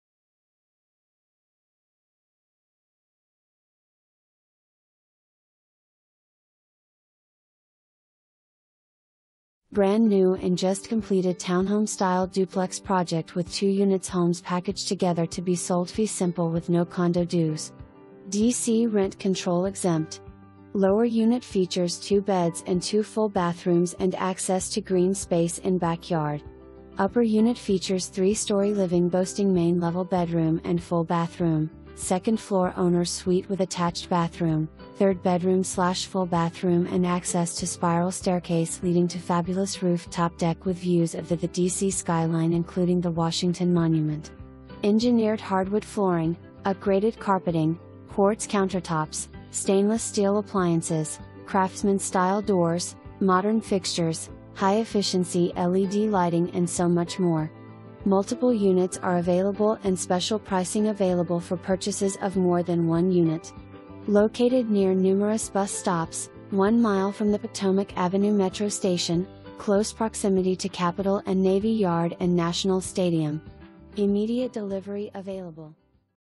2418 L'ENFANT SQUARE SE
$810,000, 2,880 SF, MLS# DCDC2086364

Brand new and just completed townhome style duplex project with 2 units homes packaged together to be sold fee simple with no condo dues! DC RENT CONTROL EXEMPT! Lower unit features 2 beds and 2 full bathrooms and access to green space in backyard. Upper unit features 3 story living boasting main level bedroom and full bathroom, 2nd floor owner's suite with attached bathroom, 3rd bedroom/full bathroom and access to spiral staircase leading to fabulous roof top deck with views of the the DC skyline including the Washington Monument! Engineered hardwood flooring, upgraded carpeting, quartz countertops, stainless steel appliances, craftsman style doors, modern fixtures, high efficiency LED lighting and so much more. Multiple units are available and special pricing available for purchases of more than one unit. Located near numerous bus stops, one mile from the Potomac Avenue Metro Station, close proximity to Capital and Navy Yard & Nationals Stadium. Immediate delivery available! Numerous projects in the area make this one of the most dynamic zones in DC! Check out attached 3D tour!

Cherish Green
Coldwell Banker Elite